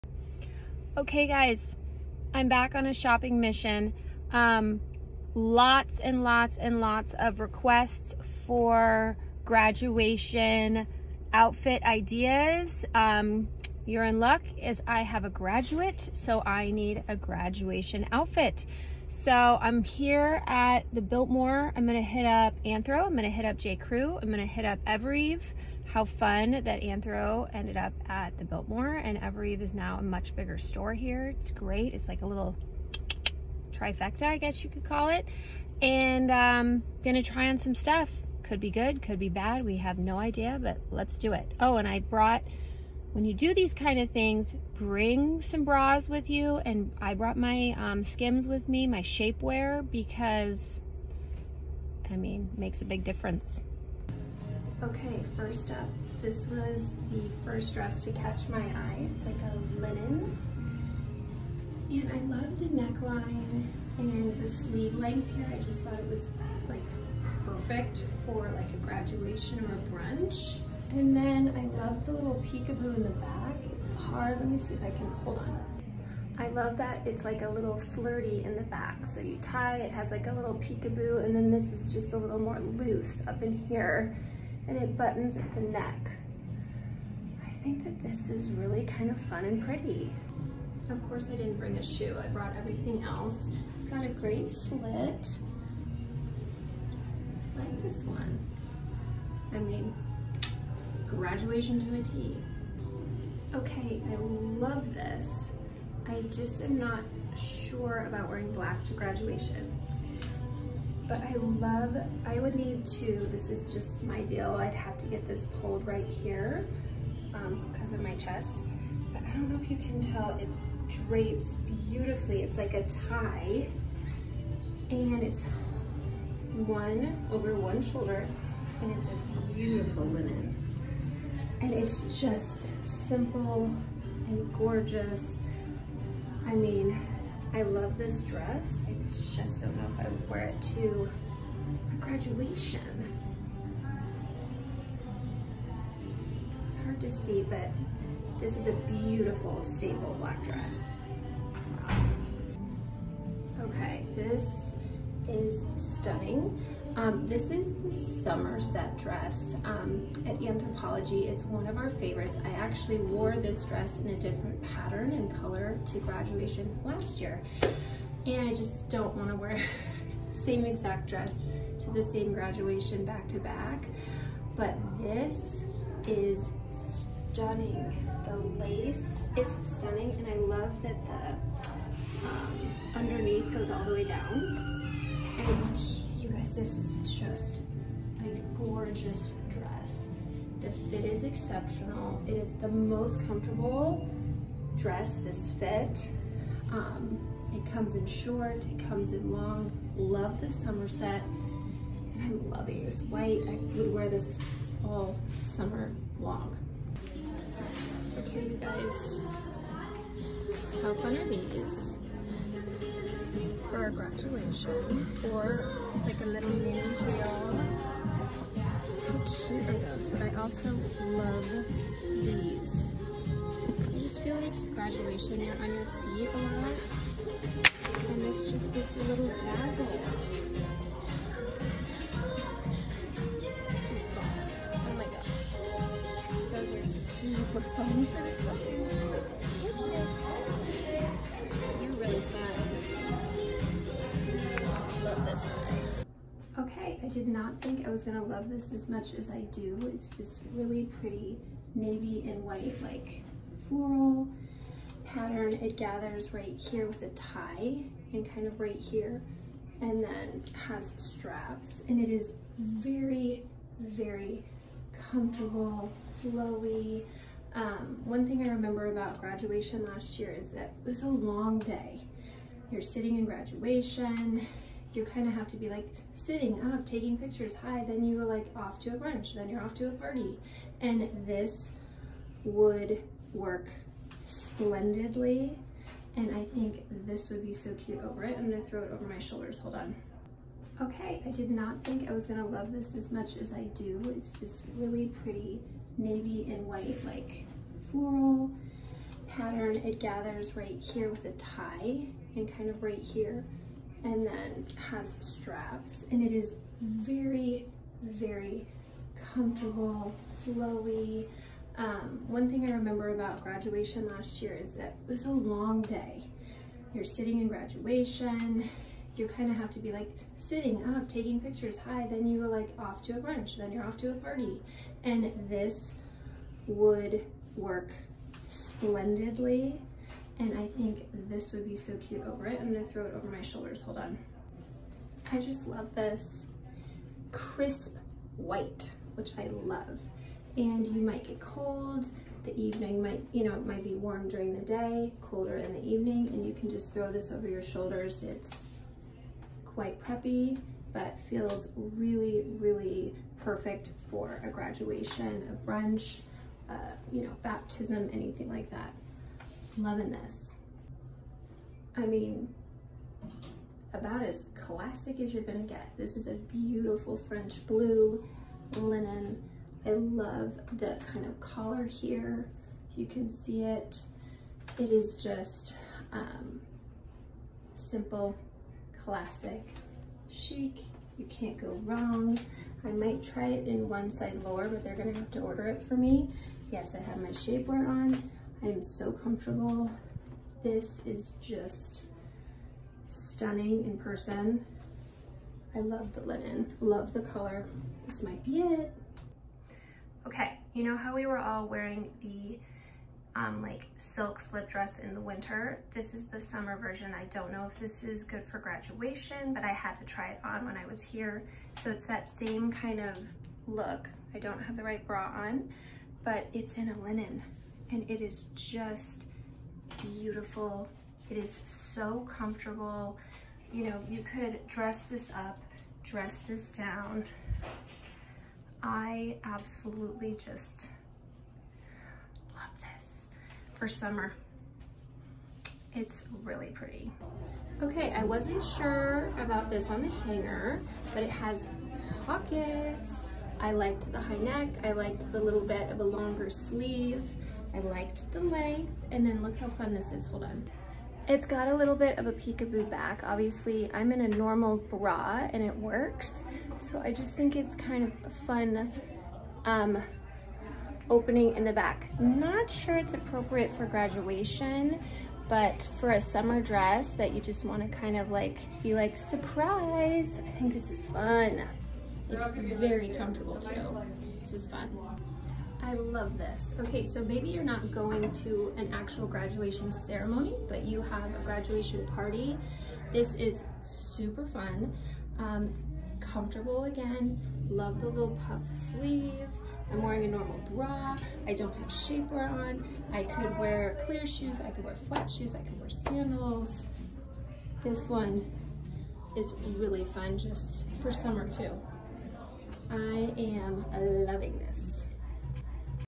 Special Occasion Dresses
Follow along as Gretchen is on a mission to find a dress to attend a graduation ceremony in!

Everything in the video is linked below